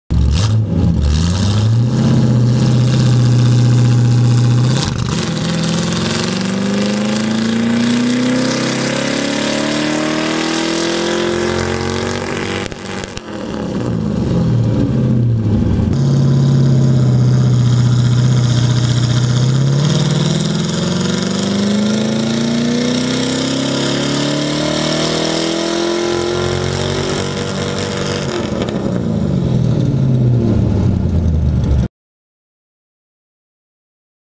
BMW swapped Ford 351w Dyno Pull
From factory this engine was rated at 210hp, 325lbft at the flywheel.
Removing all the efi intake and smog equipment Gave us 240hp, 298lbft at the wheel.  That's roughly 65hp, 20lbft gain.  Not bad for an engine from picknpull.